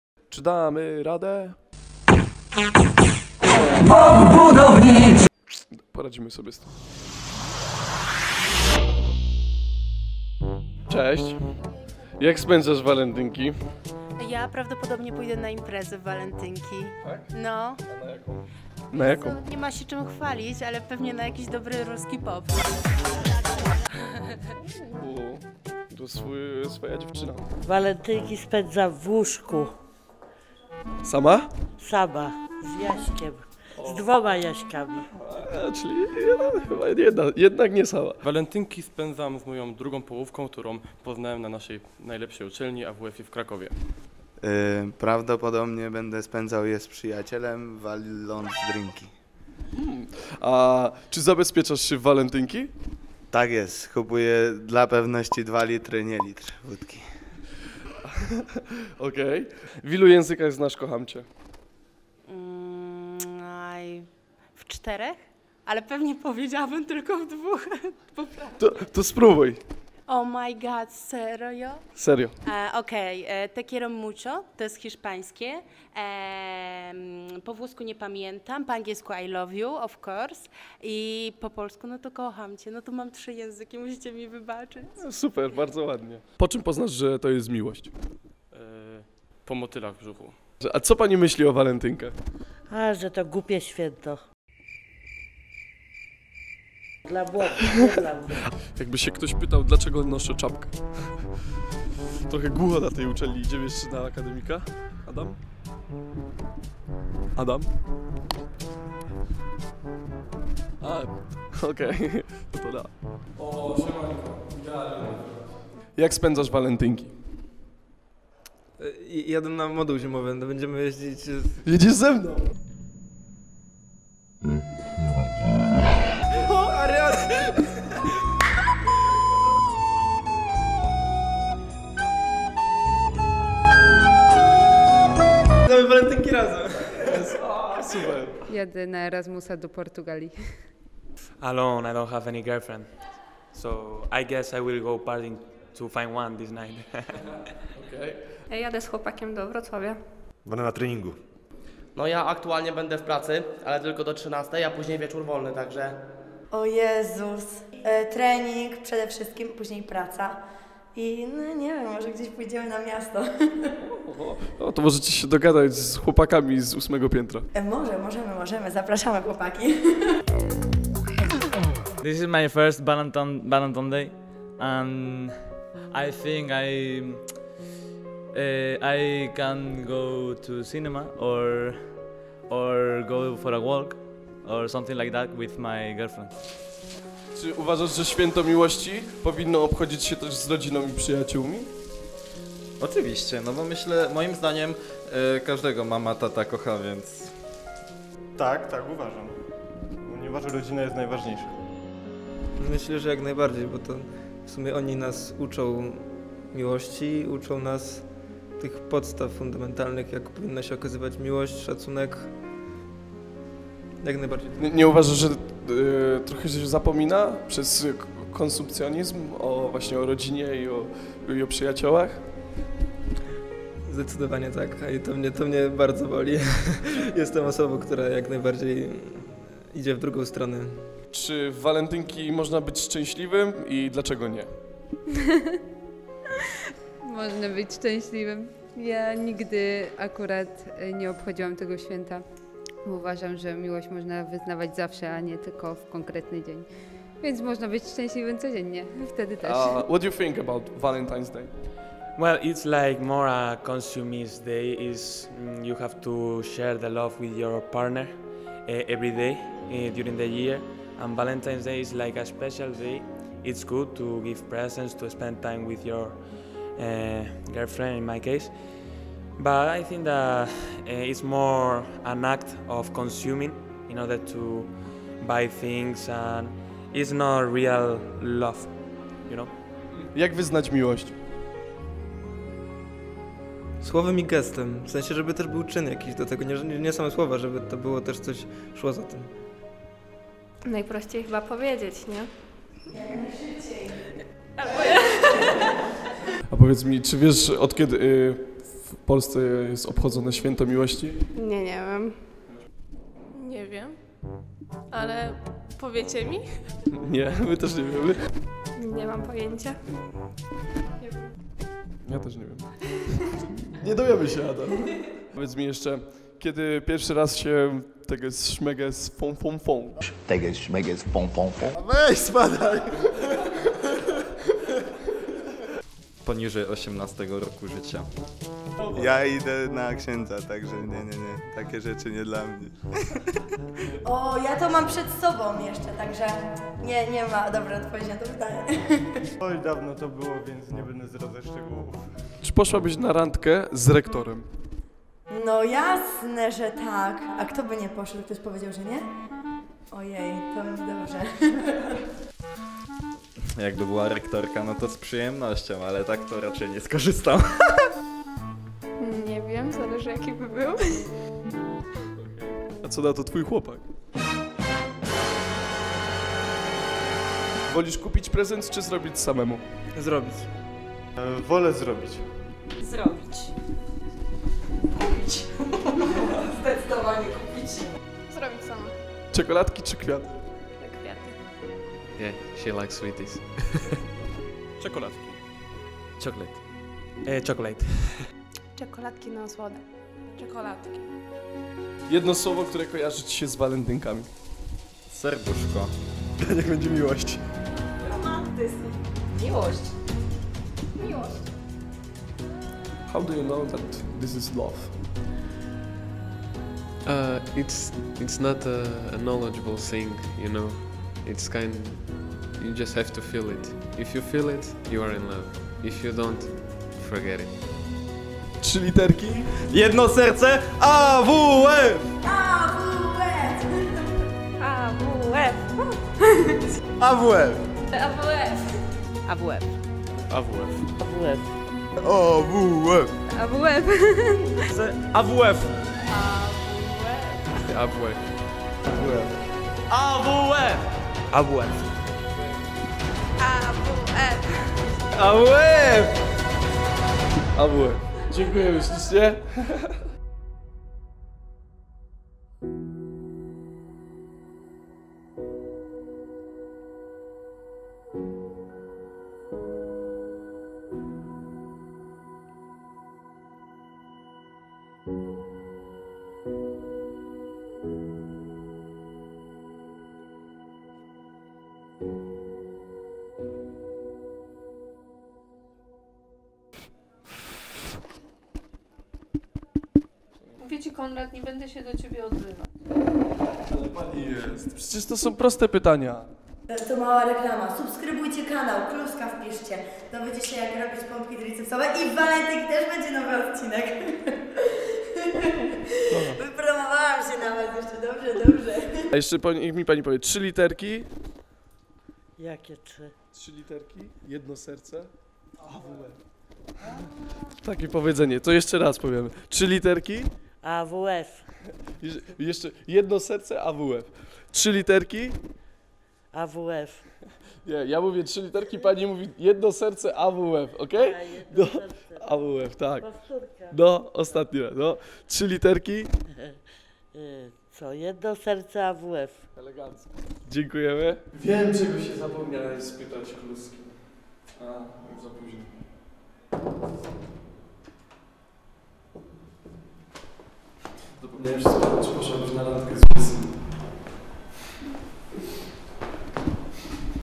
"Dzień Zakochanych" Walentynki 2020 na AWF w Krakowie
14 lutego na całym świecie obchodzimy Dzień Zakochanych, zwany potocznie jako "Walentynki". Z tej okazji ekipa AWF Channel postanowiła przepytać studentów Akademii Wychowania Fizycznego w Krakowie jakie mają plany na ten dzień. Jakie były odpowiedzi? Przekonajcie się sami!

PROWADZĄCY: Konrad Ziemianin
ZDJĘCIA: Adam Nowak
MONTAŻ: Adam Nowak
GRAFIKA: Daniel Pastuszka